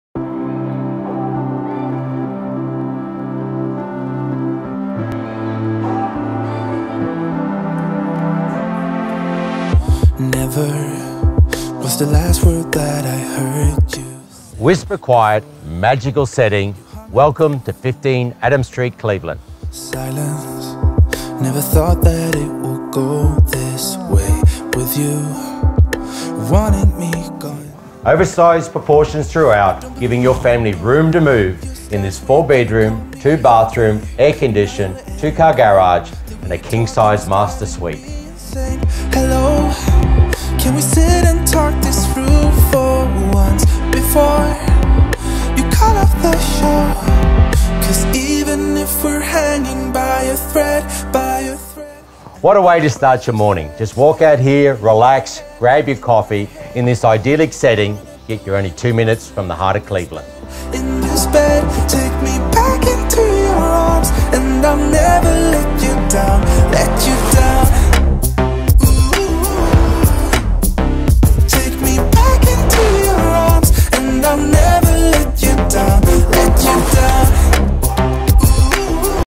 15 Adam Street, Cleveland
The Winning Combination - Position, Privacy, Space & Side Access!

The peaceful nature reserve that borders this beautiful 4 bedroom home ensures the atmosphere here is one of sheer privacy and tranquillity.

Combine the idyllic position with a generous, oversized floor plan and there's plenty of room for a family to spread out and enjoy relaxed, easy living and fantastic outdoor entertaining all year round.

All of this, and you're only 5 minutes from all that Cleveland has to offer - Harbourside fine dining, cafes, shopping centres, train station, hospital and popular Cleveland High School. You will need to be quick with this one, to avoid missing out on a great buy!